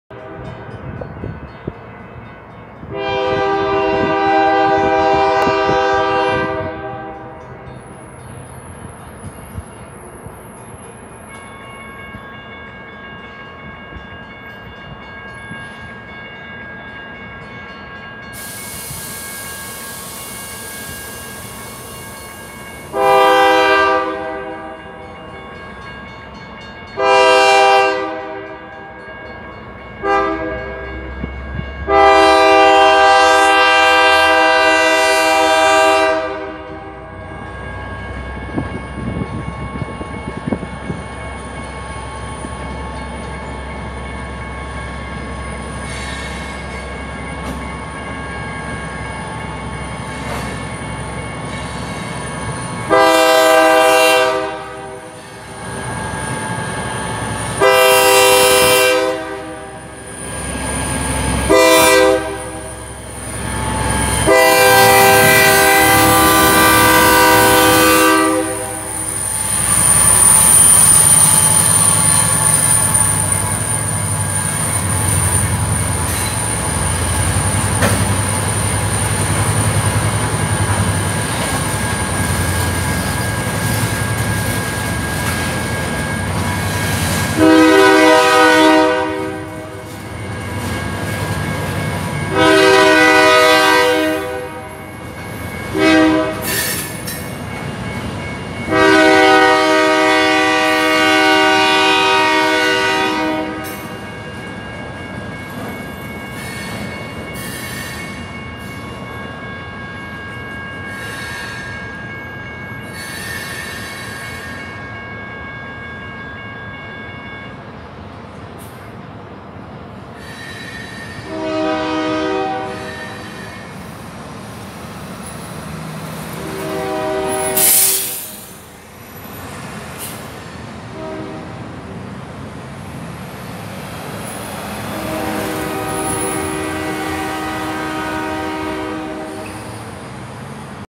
Friday afternoon Run part 2...Portland & Western 2311 crossing W main st in downtown Hillsboro
Portland & Western 2311(old Santa Fe paint scheme) crossing West main street and SW Washington st in downtown Hillsboro hauling 6 full centerbeam lumber cars from Hampton lumber mill in Banks Oregon.